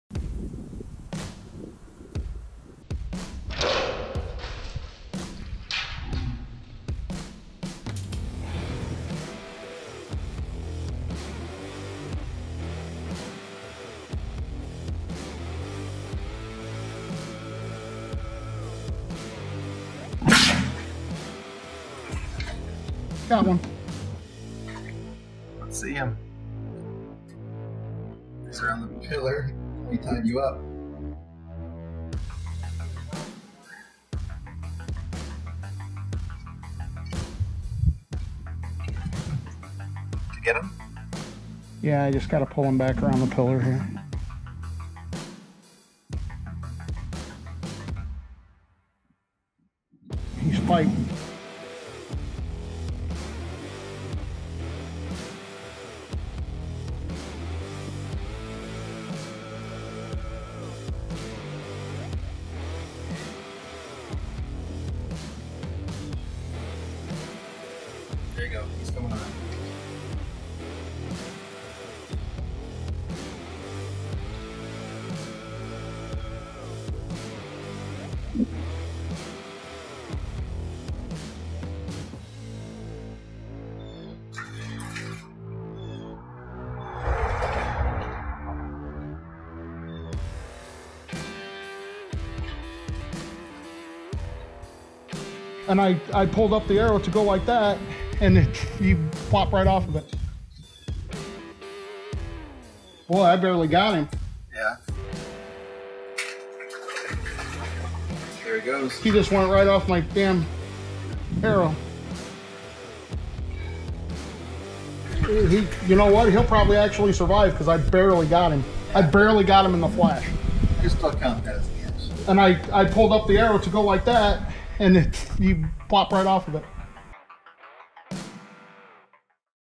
The Last Fish of the Season, Bow Fishing Under the Bridge
It's the last fish of the season, and thanks to the cold weather we're able to get under the bridge where the carp are hiding. Turns out there are dozens of them around those pillars and they'll be ready to fish out next year. In this video, Dan is using his new reel thanks to FishZilla breaking his old one. Unfortunately, this reel has been giving Dan problems due to the line not wrapping tightly, so he'll be replacing it over the winter with a better one. Expect a review for this reel sometime in the future.

Always remember to be cautious with your gear to prevent injuries!